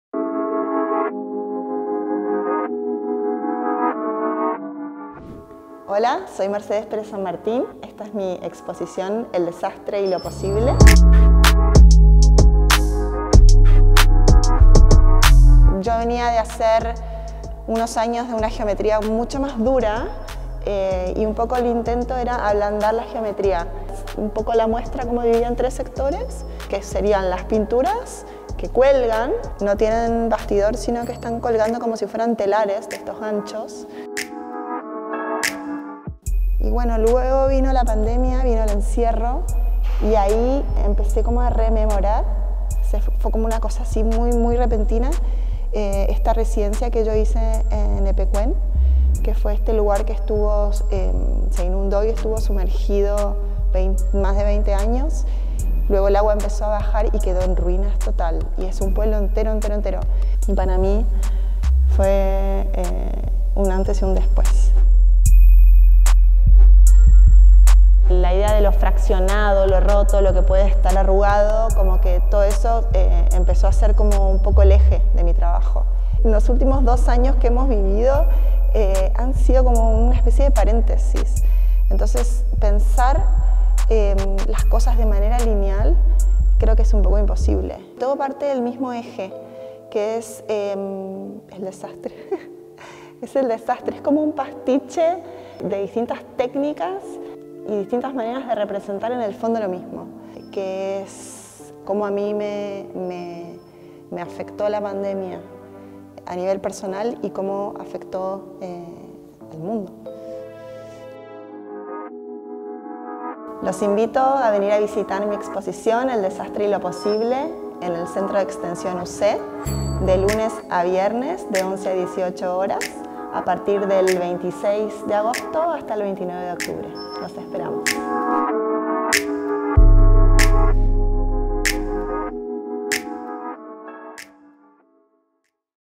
“El desastre y lo posible” llega al Centro de Extensión UC
Esta muestra presencial, nacida desde la la fragmentación del tiempo en la pandemia y las ruinas de Epecuén, abre presencialmente desde el 26 de agosto al 29 de octubre en Alameda 390.